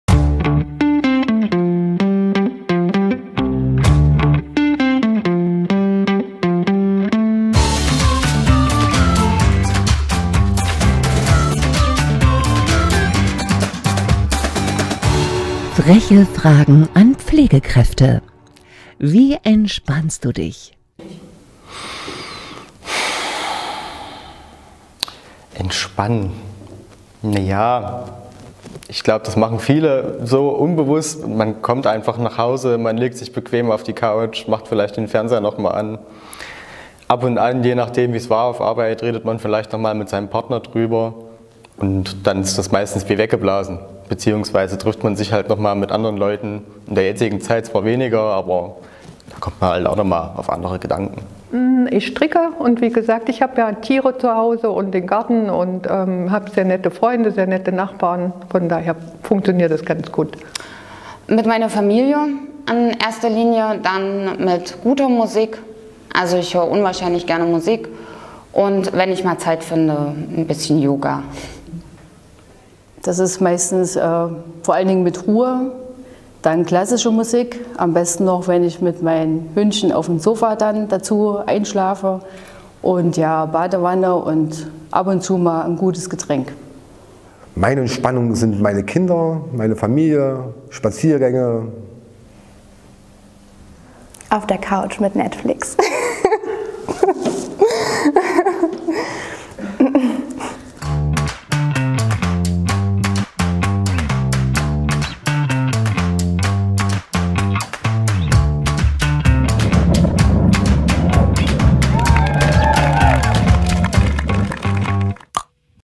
Folge 6/14: Wie entspannst du dich?
Freche Fragen an Pflegekräfte – Über Arztserien, Polizisten und ein dickes Fell ...

Wir fragen in unseren „Frechen Fragen an Pflegekräfte“ u. a. nach dem Berufswunsch in der Kindheit, ob sie lieber Mediziner geworden wären, was Patienten über sie sagen oder welche Lieblingsarztserien sie schauen. Mit diesen Videos möchten wir zeigen, wie humorvoll, nachdenklich, zupackend und unterschiedlich die Menschen sind, die bei uns in der pflege arbeiten. Außerdem sind viele Folgen sehr unterhaltsam.